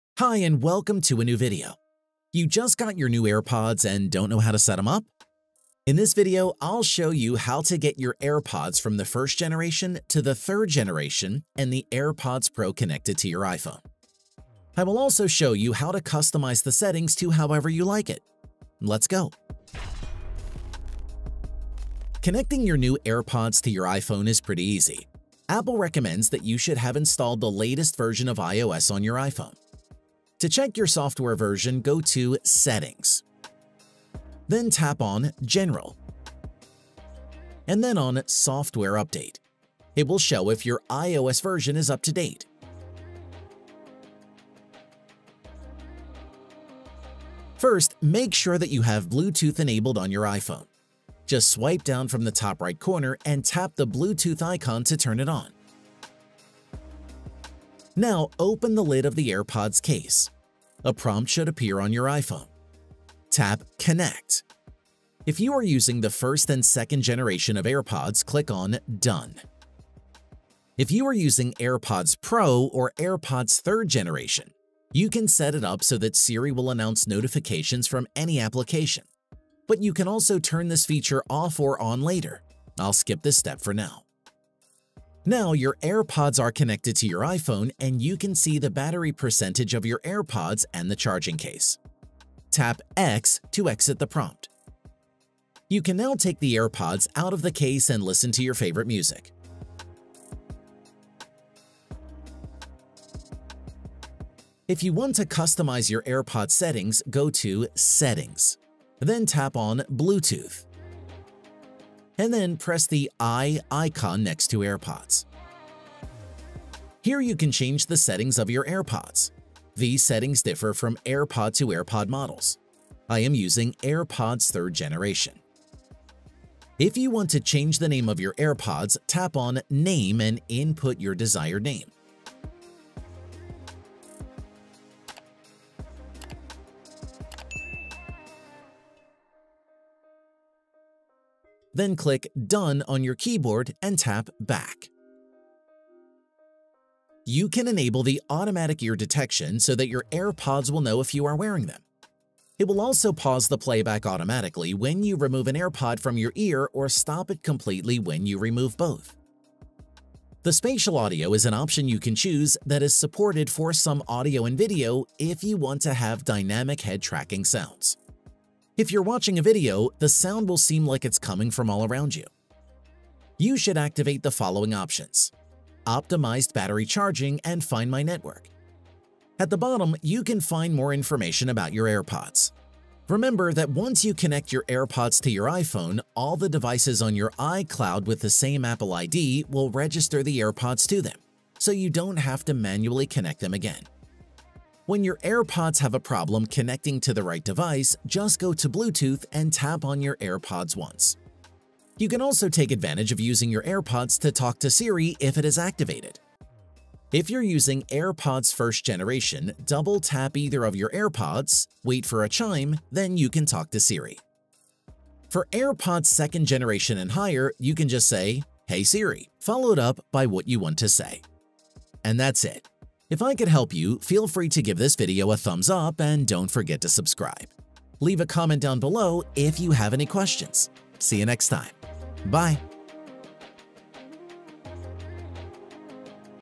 How to Connect & Setup AirPods to iPhone or iPad!
Just got your AirPods and don't know how to connect it to your iPhone? In this video I'll show you how you can setup any model from AirPods 1st Generation to 3rd generation and AirPods Pro and how you can customize it to your preference. Yours Jigxor

Timestamps:
00:00 | Setting up your AirPods
00:47 | How to do it
01:43 | Customizing Settings
03:21 | Using Siri on AirPods

Video: 143